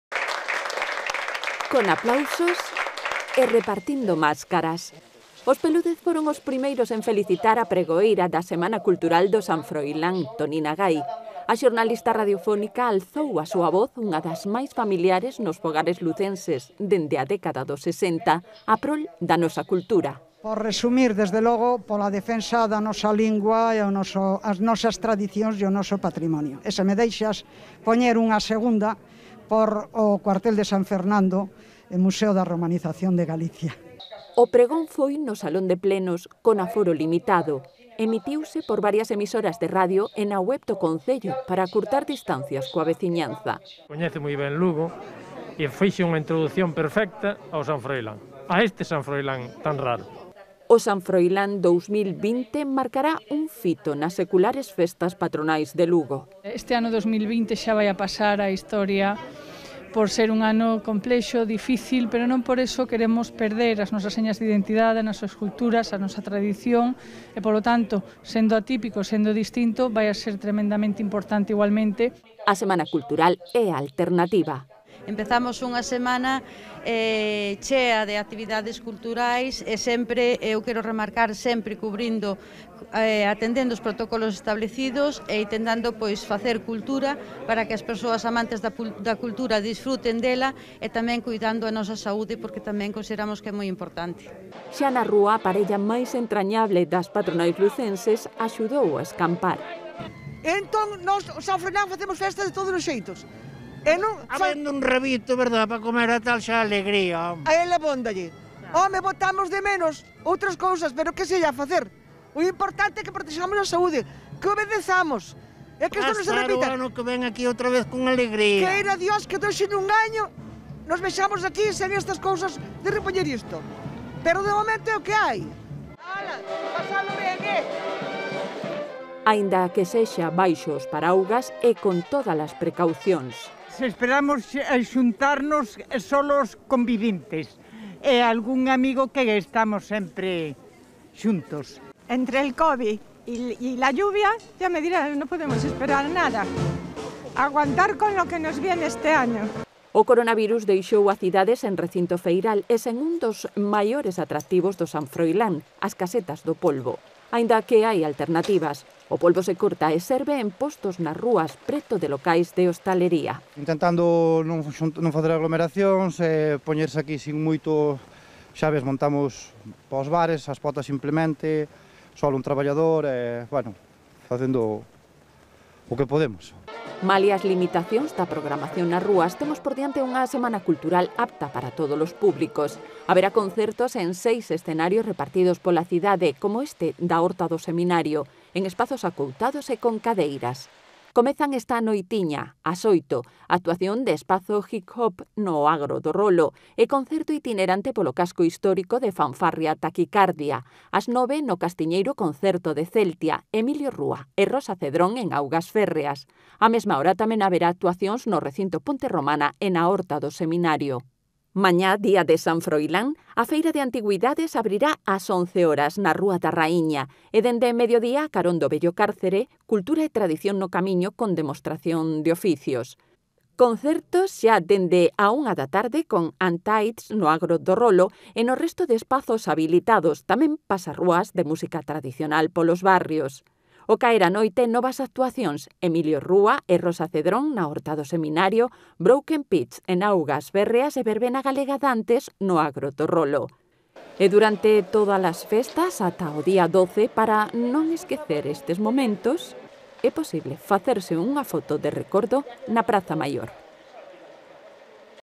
Comeza o San Froilán 2020
Con aplausos e repartindo máscaras, os Peludez foron os primeiros en felicitar á pregoeira da Semana Cultural do San Froilán Tonina Gay